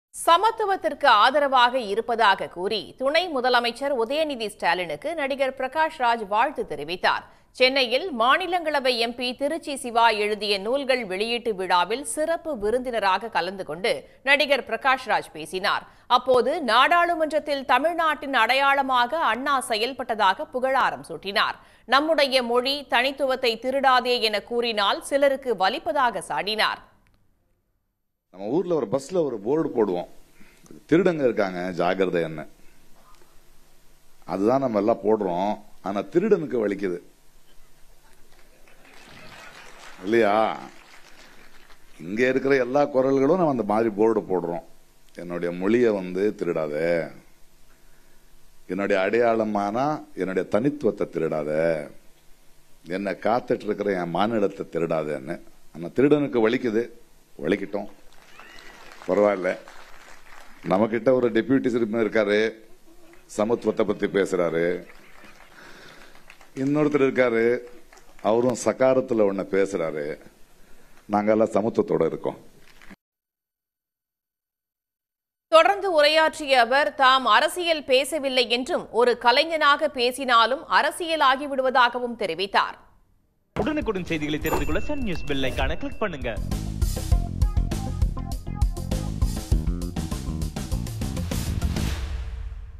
Prakash raj Speech on Thirupathi laddu Controversy | Sun News | Pawan Kalyan
திருடனுக்கு வலிக்கிறது திமுக மேடையில் தீப்பறக்க பேசிய பிரகாஷ் ராஜ்